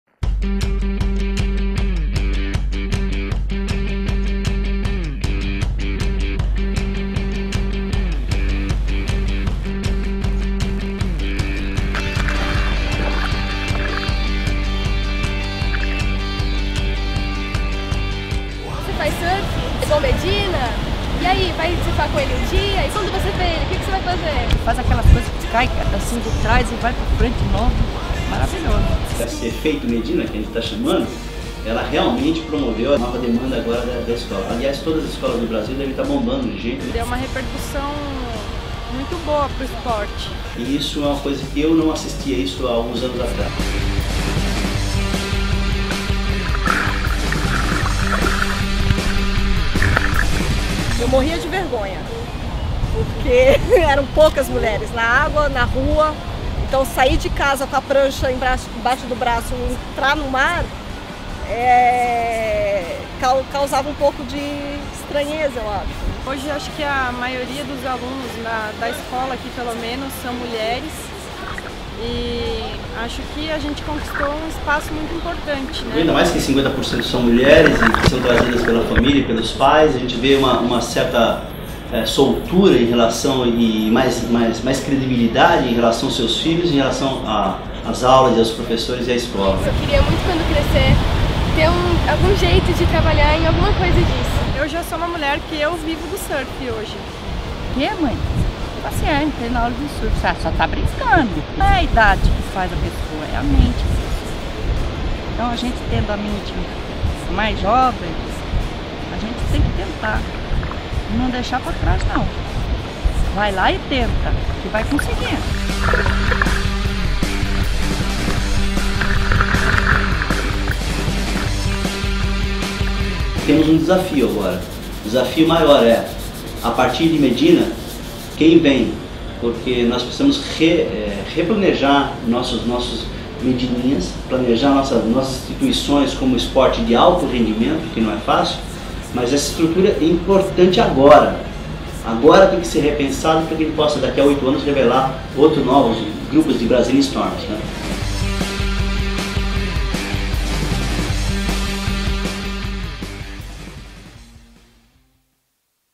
'Efeito Medina' leva mulheres para o surfe
Após a conquista do primeiro título mundial de surfe, a procura pelo esporte cresceu e público feminino aumentou nas aulas das escolas públicas de surfe de Santos, litoral de SP